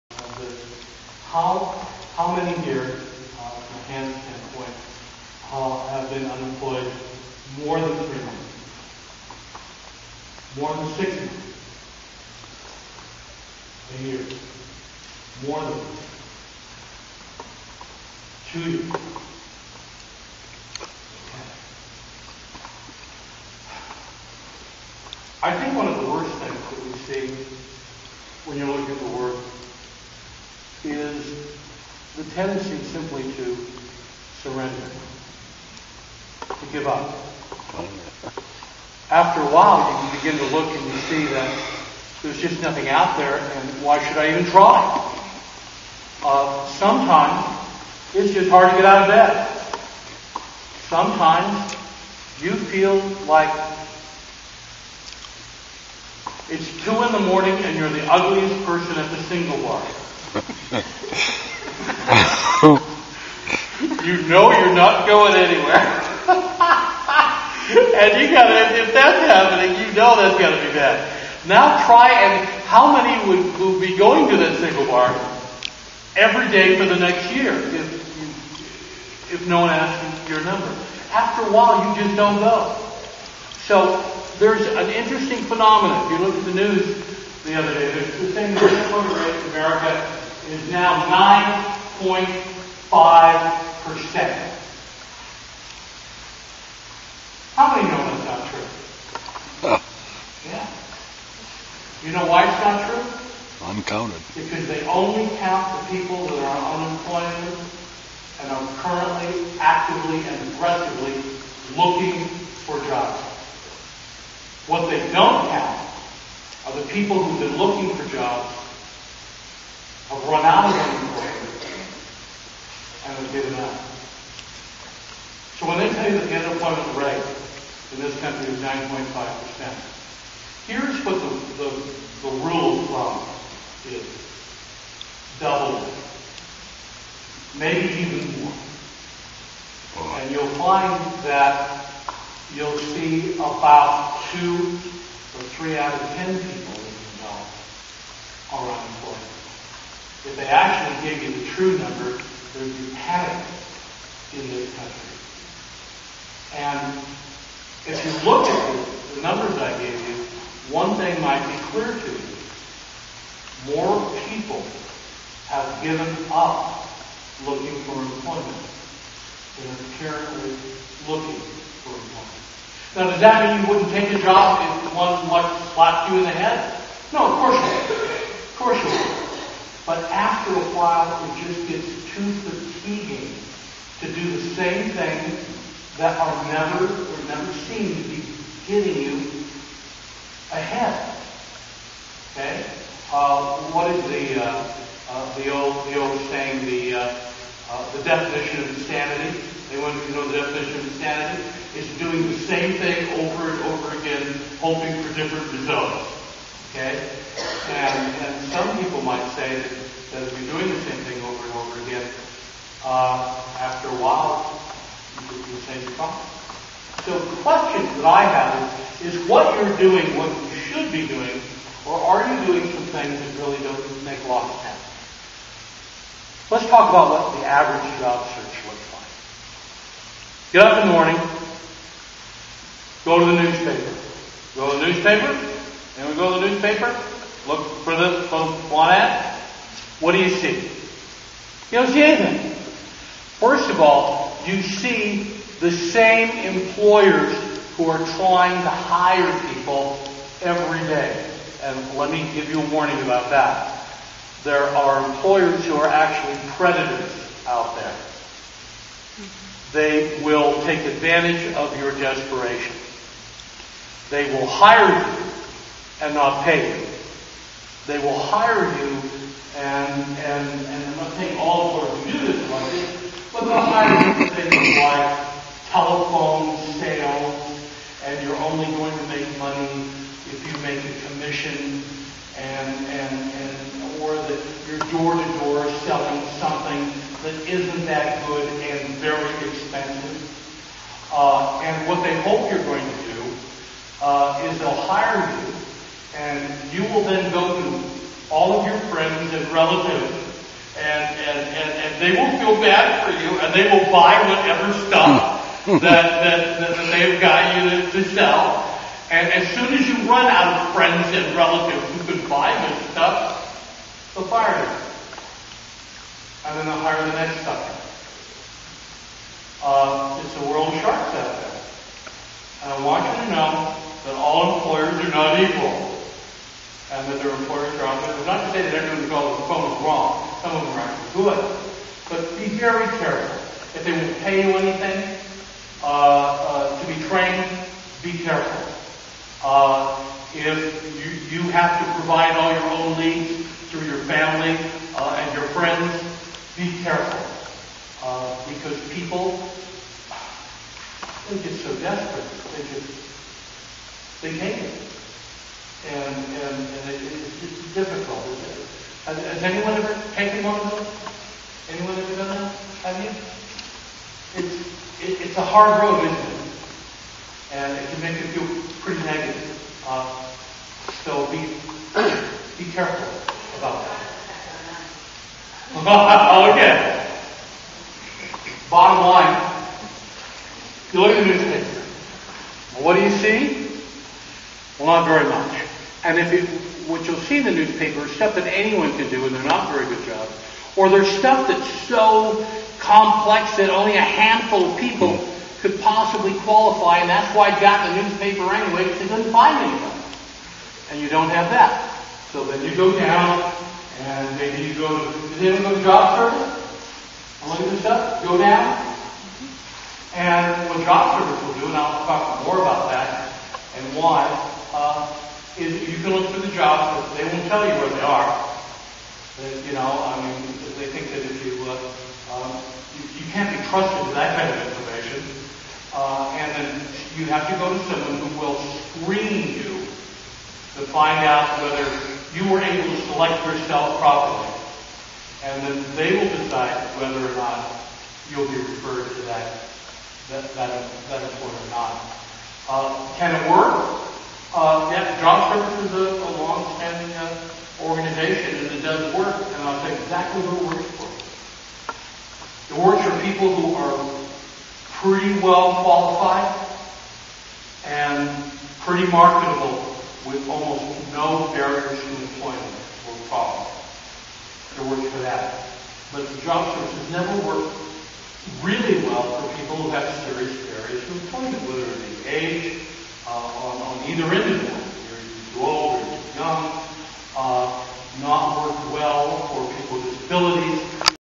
pt 3 the seminar on job search
dr. richard gives advice on how to look for work